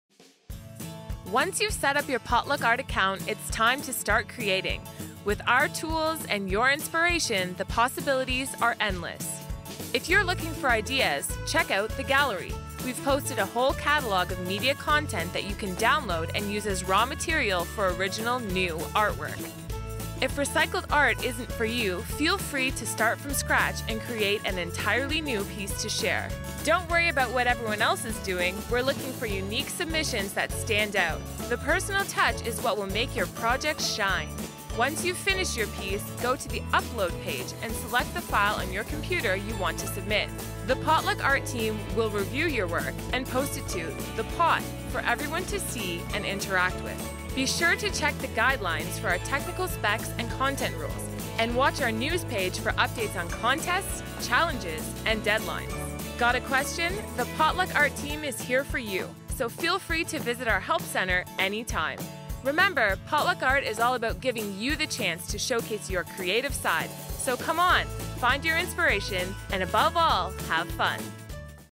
Potluck Art - Getting Started
Potluck Art, a new online platform that gives Canadian students the opportunities to create their own unique digital media content, launches July 1, 2012. Here's a helpful guide to getting started and making the most of the Potluck Art experience.